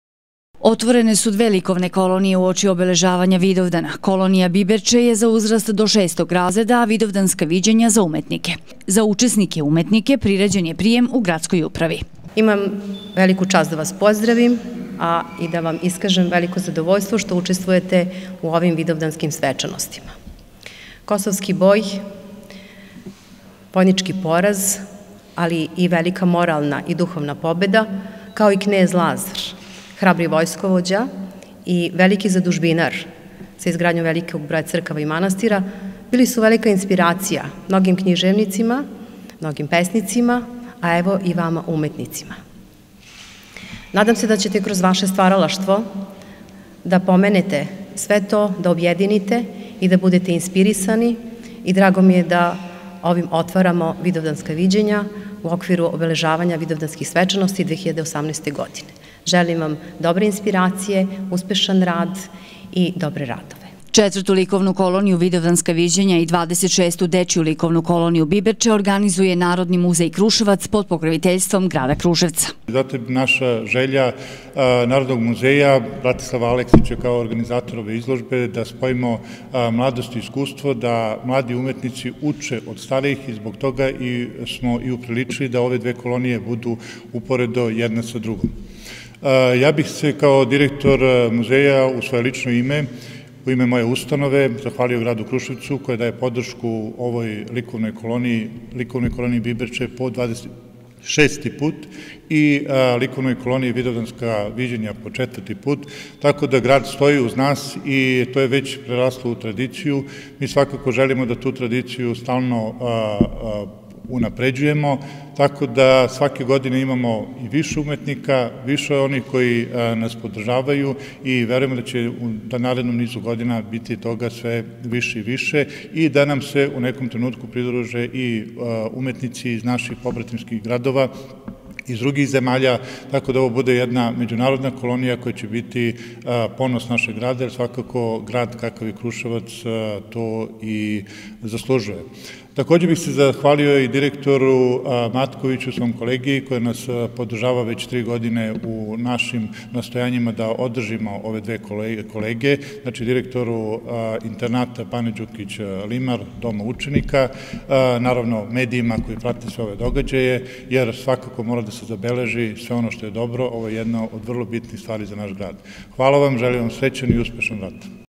LIKOVNE KOLONIJE U SUSRET VIDOVDANU 2018
U okviru Vidovdanskih svečanosti danas su otvorene dve umetničke likovne kolonije "Biberče", za uzrast do 6. razreda i Vidovdanska viđenja za afirmisane umetnike. Likovne kolonije organizuje Narodni muzej Kruševac.